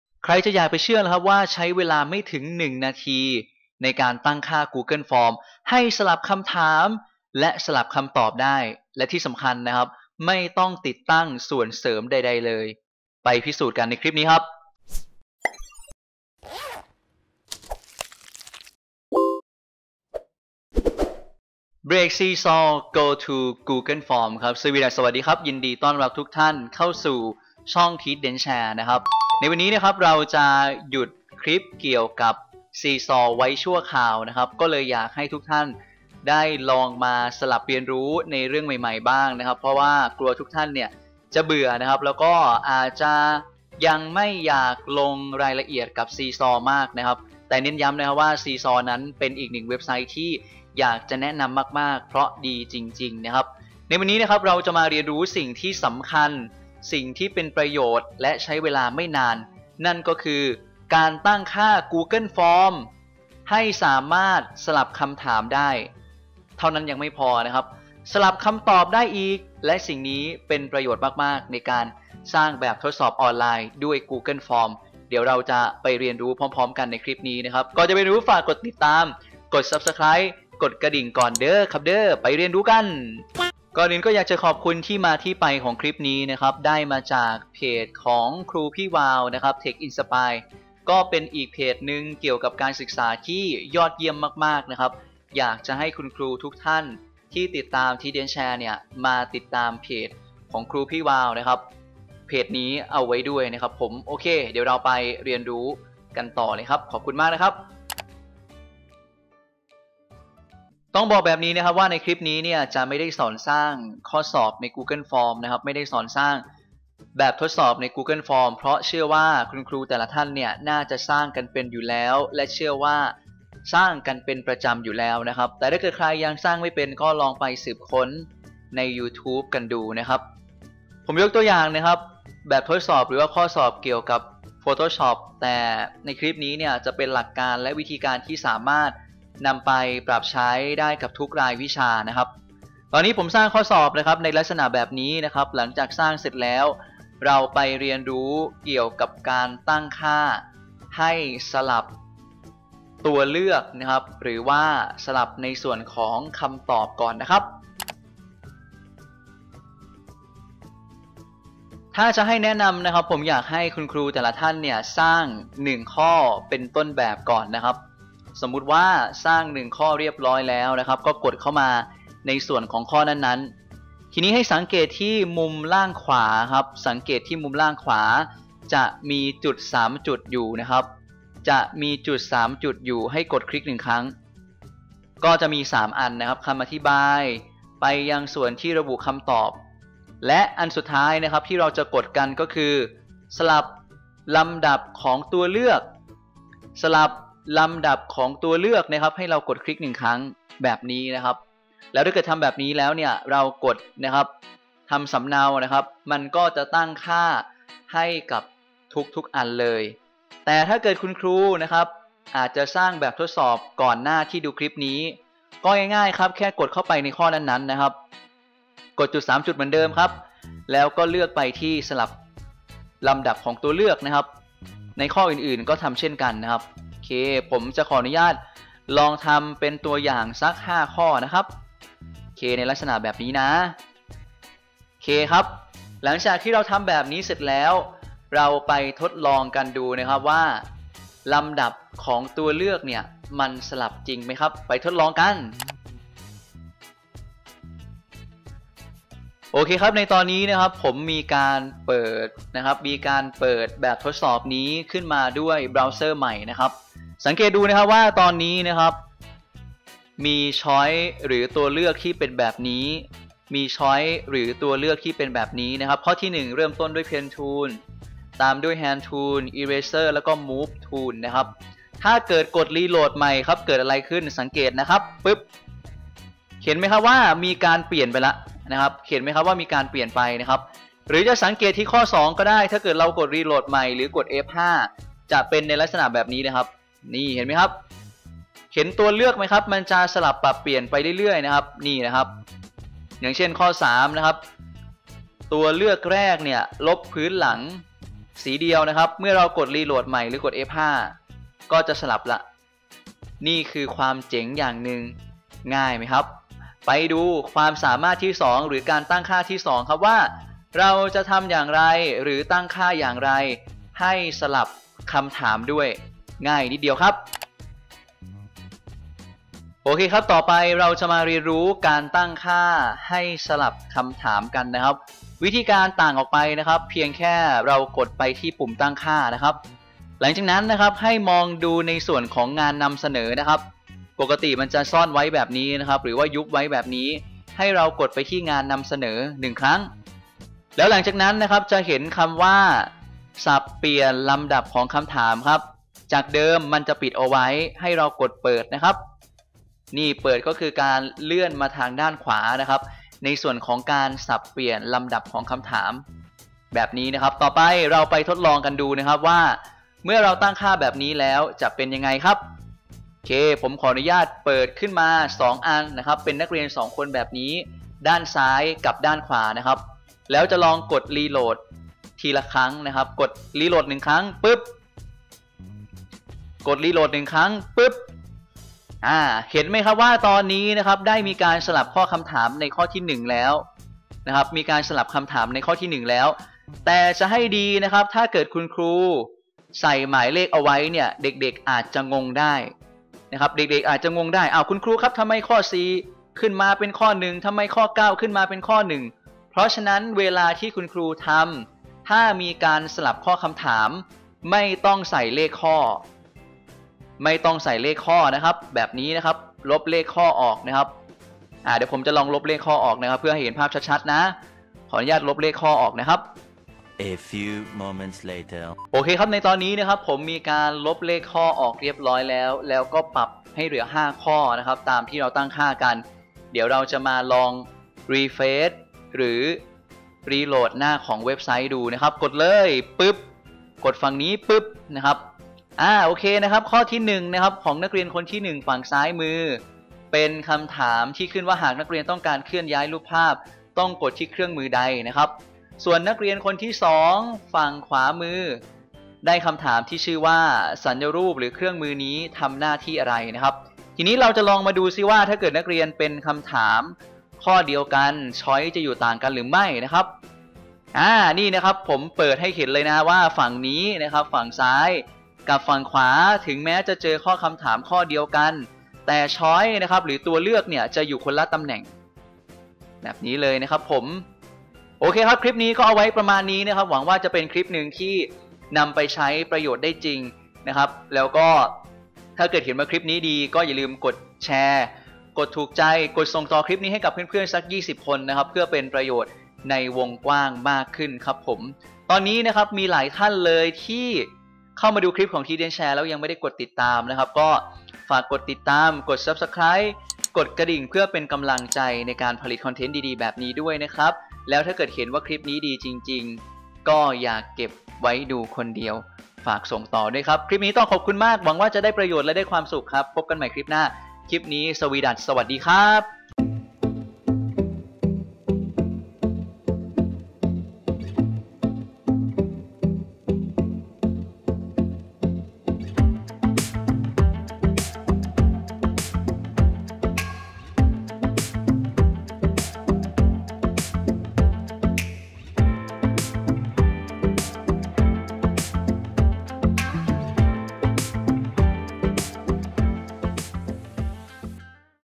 การตั้งค่าให้แบบทดสอบใน Google Forms สลับคำถามและคำตอบ (ง่าย ๆ ไม่ถึง 1 นาที)
คลิปวิดีโอนี้ : คลิปวิดีโอนี้ทุกท่านจะเรียนรู้การตั้งค่าให้แบบทดสอบใน Google Forms สลับคำถามและคำตอบ ทำอย่างไร ไปดูกัน